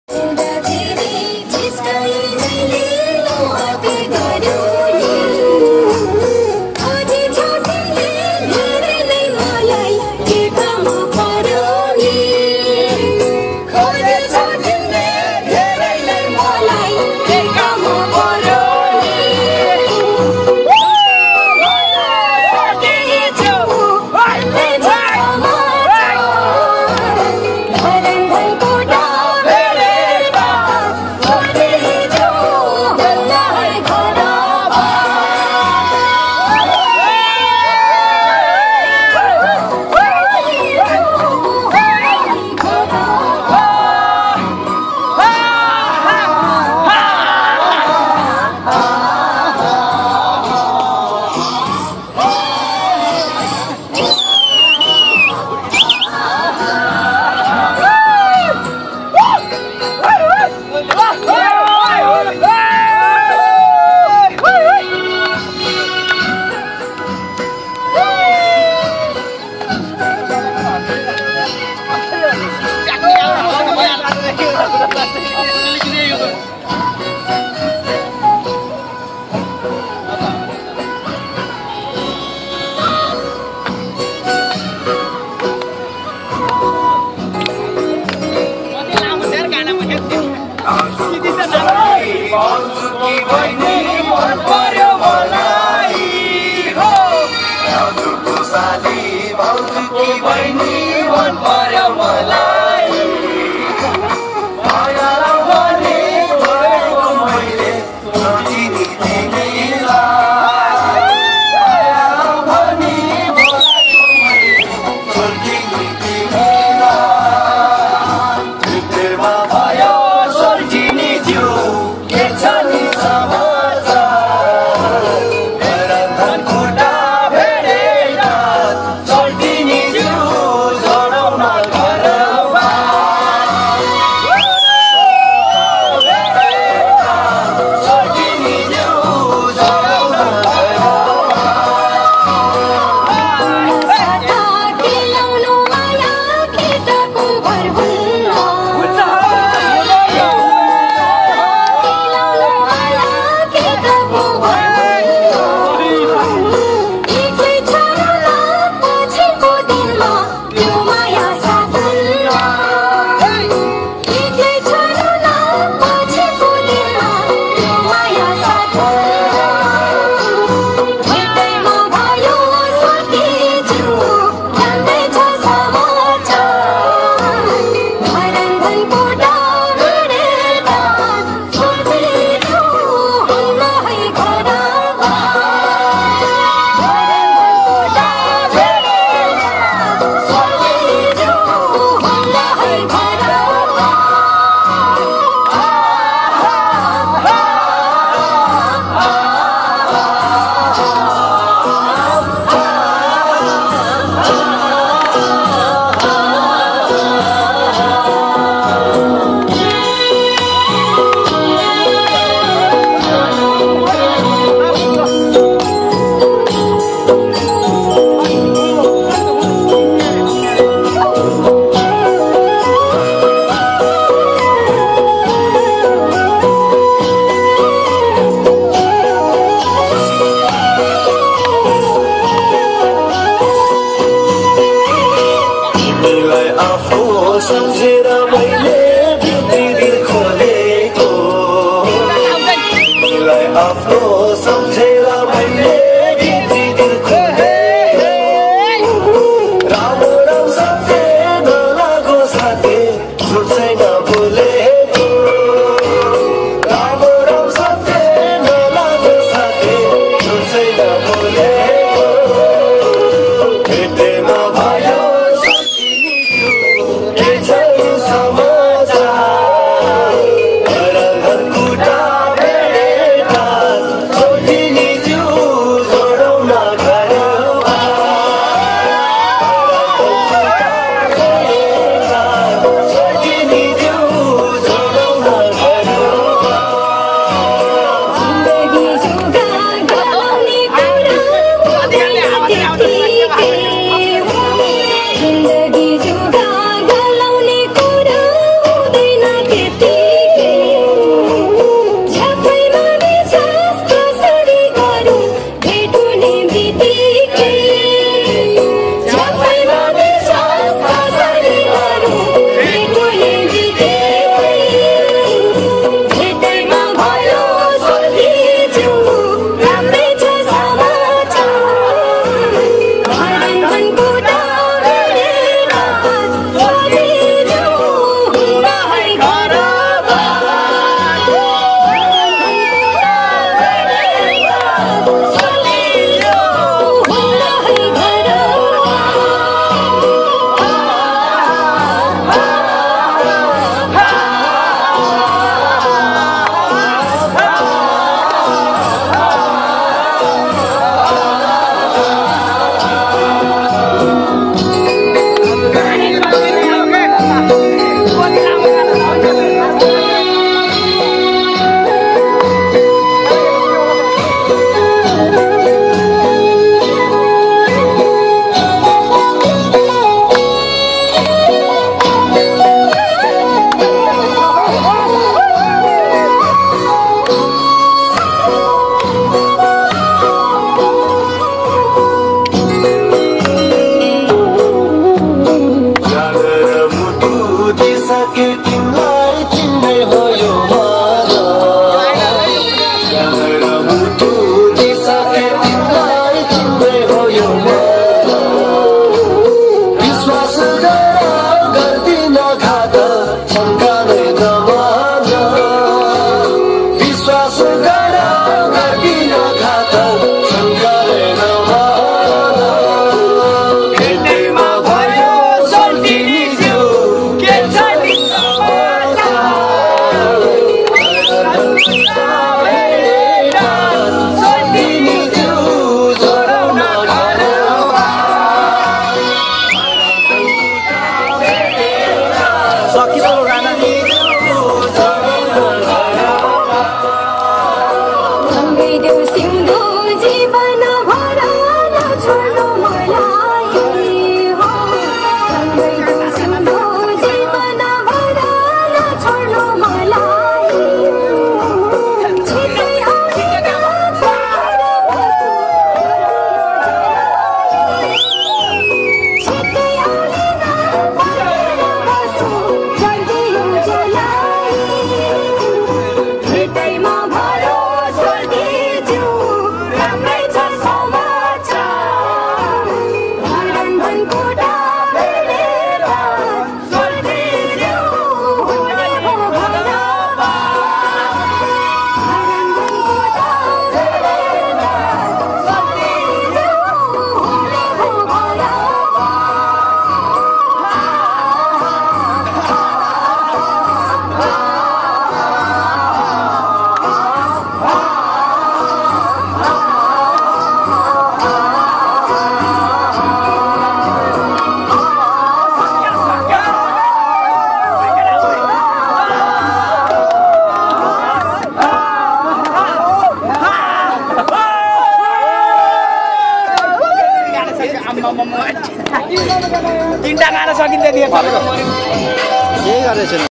EID Holiday Visit in home side/होम साईड को नचाई ईद को छुट्टी लाई सदुपयोग
Cameras Man : Ram Bantwa
Cover Dance by : Sanjog Rai , Pramod Regmi , Kumar pradhan, Shyam Karki , Mekha Karki , Jiban baraili , Chudha Mani Saphkota.